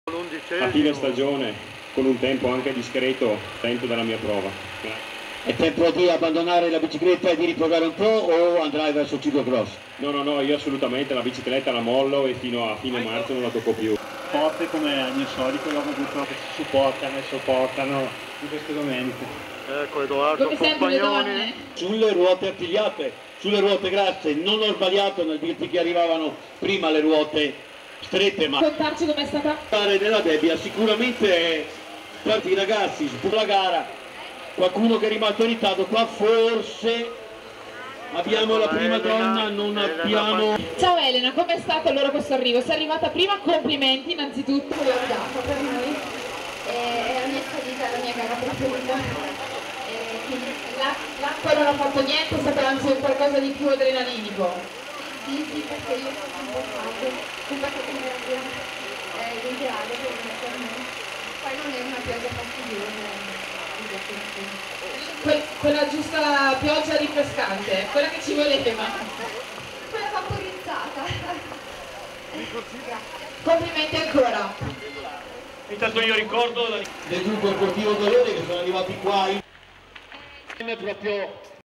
VTS 05 1
ALESSANDRO LEALI ODOLESE  CRONOSCALATA M,MADDALENA BICISU STRADA E MTB,ARRIVIV ZITO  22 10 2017  INTERVISTA A ELENA PANCARI  A ANDREA RIGONI